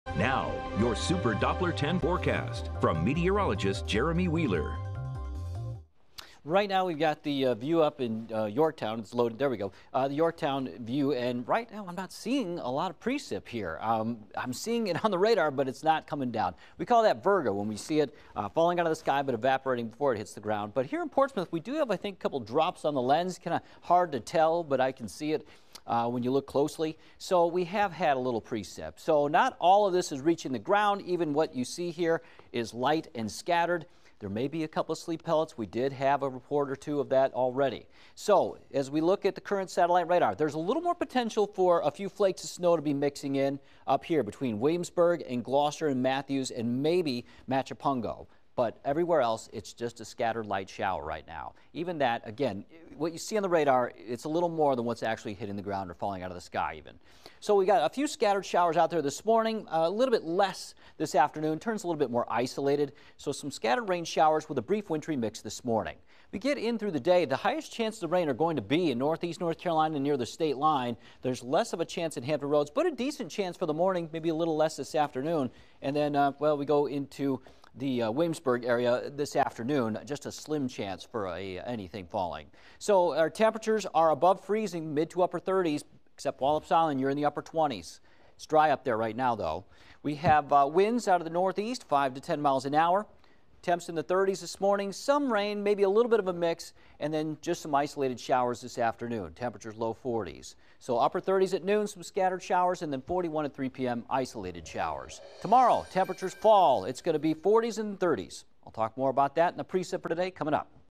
WAVY Weather Morning Update | Feb. 2, 2023
Super Doppler 10 Meteorologist Jeremy Wheeler has the latest weather forecast for the Hampton Roads viewing area.